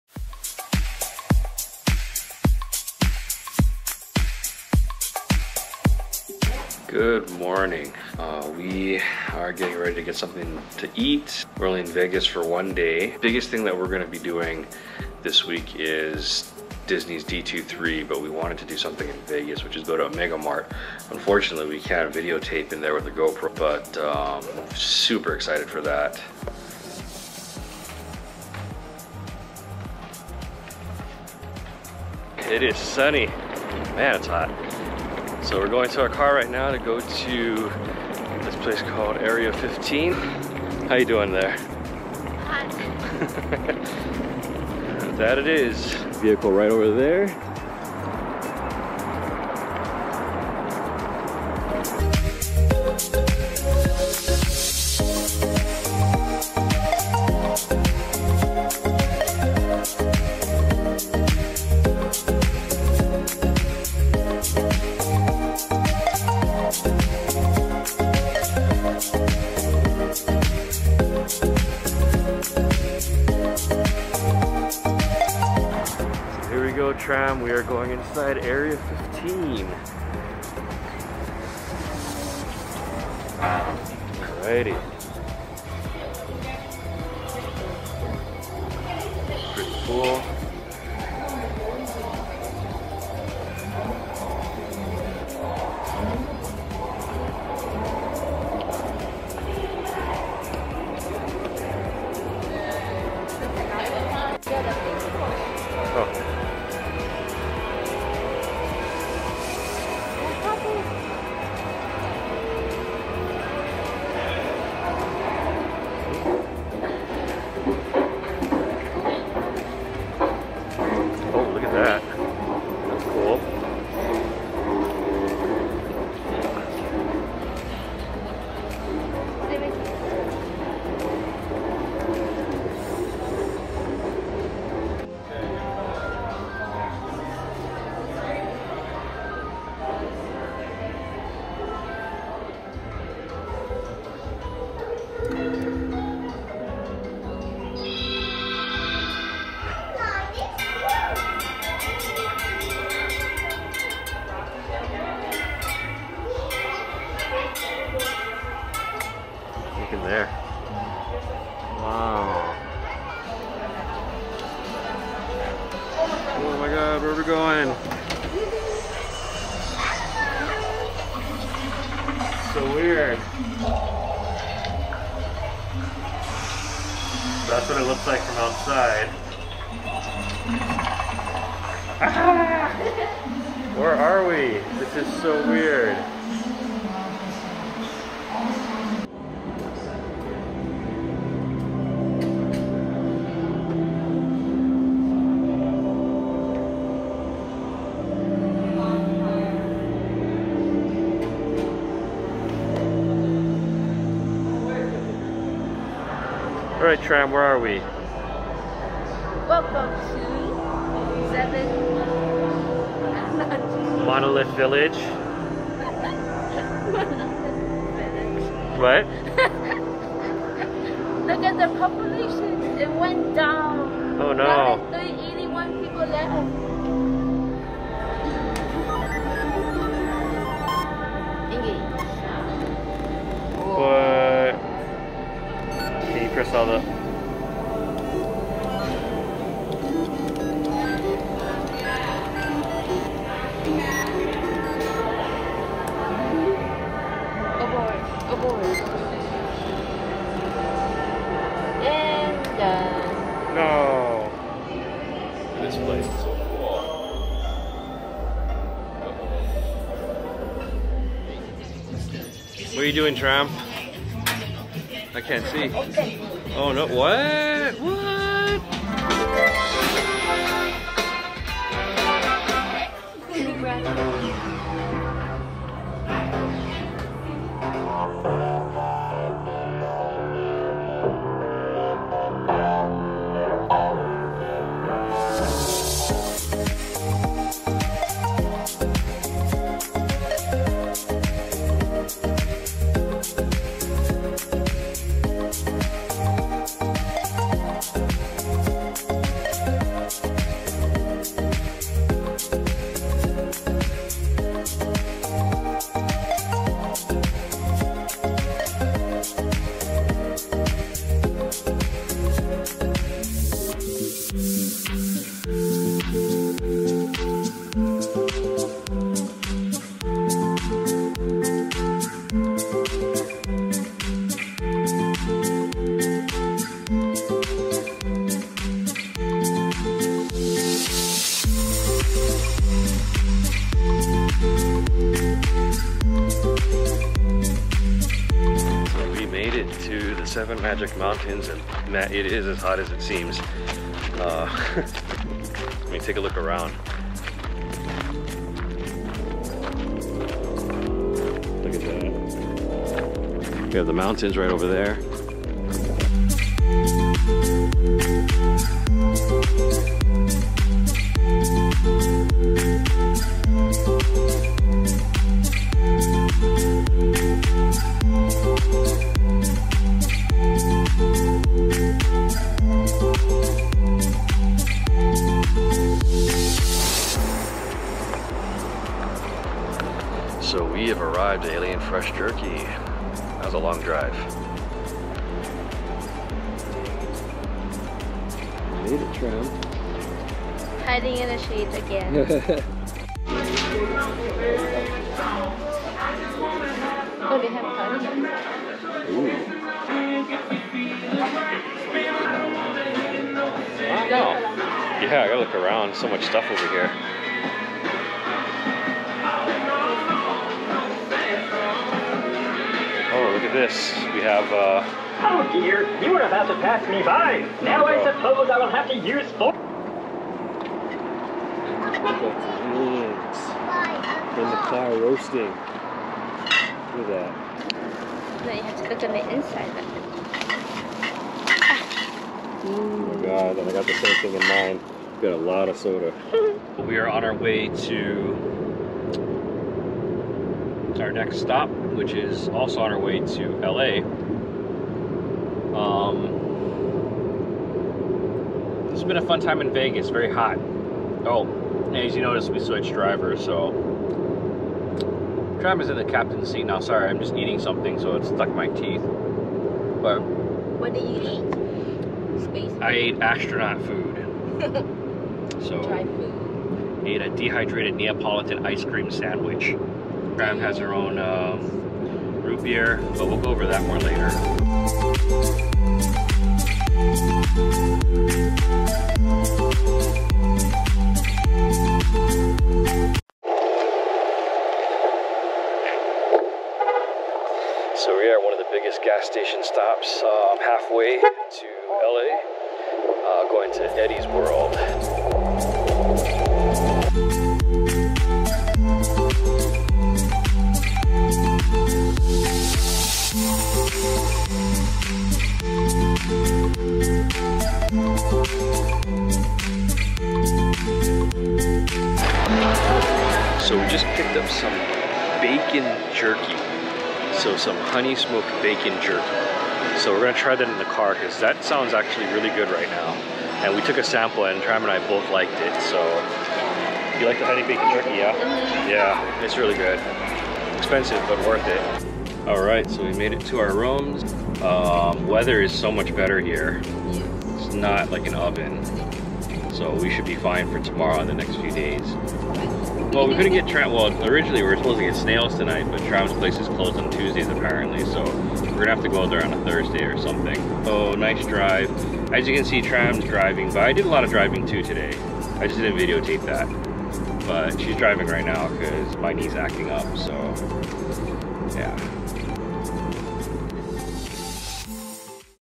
Omega Mart, 7 MAGIC MOUNTAIN, Alien Jerky, Eddie's World, and LA
Our stops on the way to LA